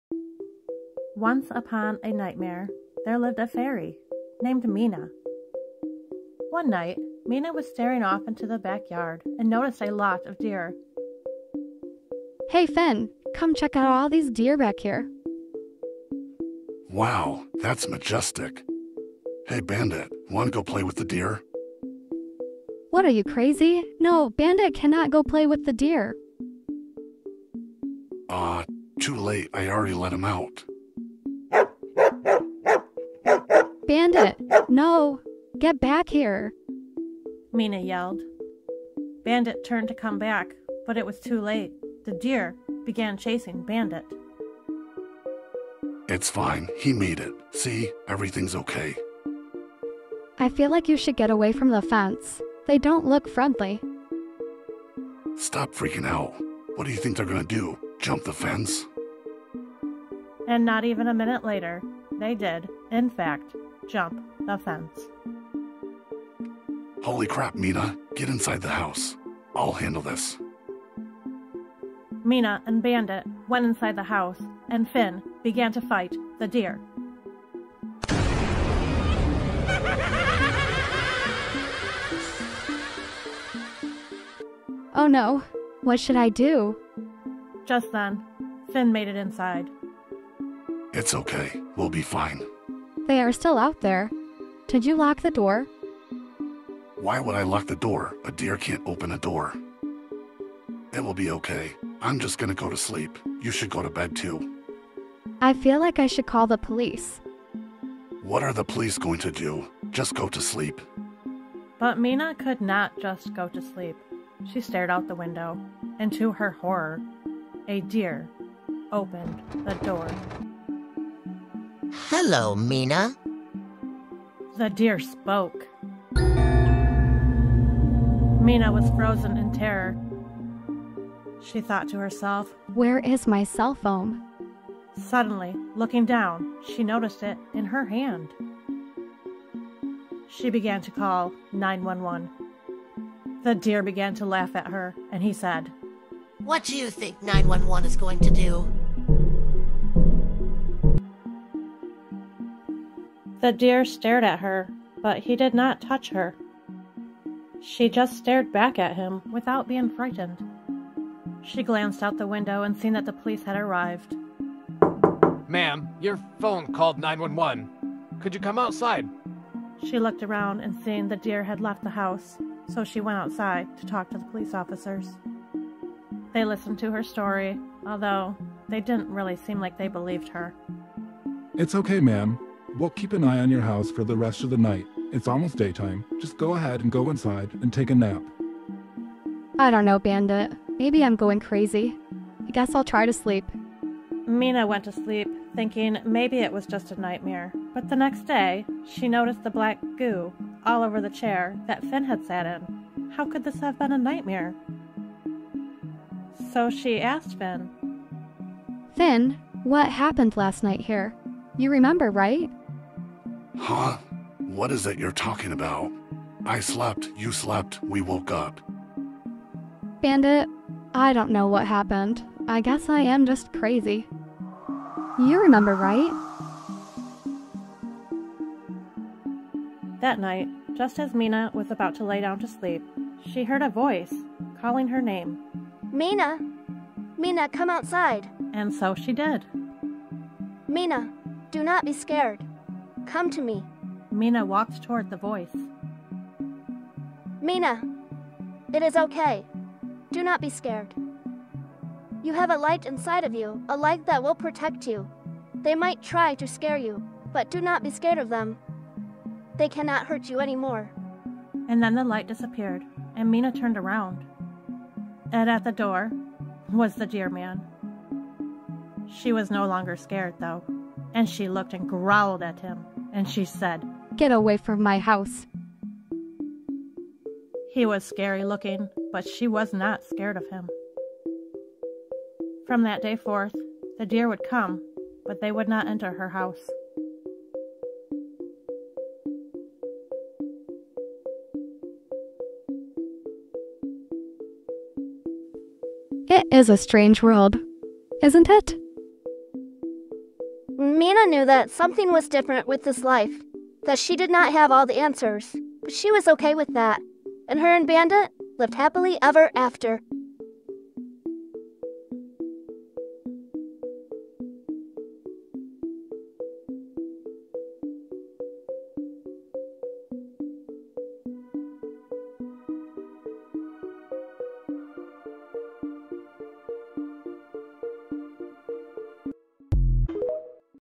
Once upon a nightmare: SHAPESHIFTING evil DEER
Mina, Finn & Bandit fight off shapeshifting evil deer.  Mina gets a message from a bug fairy that informs her she will be ok even though the evil deer continue to torment her.  Mina learns that the light inside of her can not be taken away or dimmed no matter how hard her enemies try.